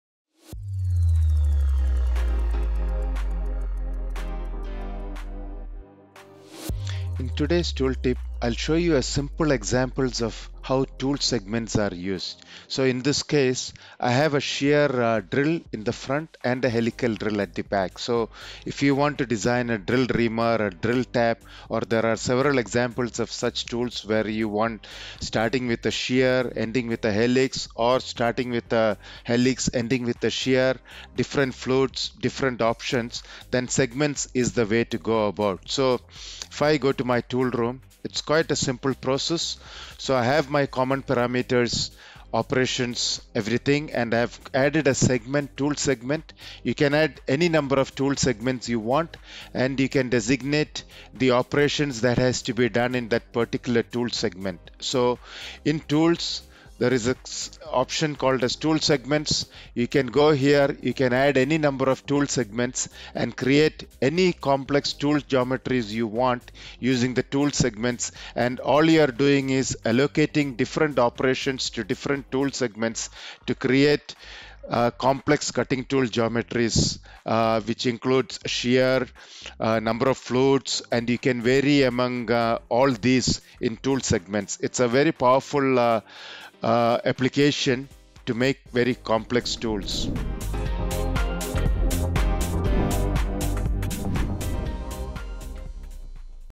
Tool Tip Tuesday #216 - Complex Tools Using Tool Segments
In this ANCAToolTipTuesday we look at how tool segments are used to create complex tools.

We hope you found this tip useful and would love to hear any suggestions for tips you would like us to cover.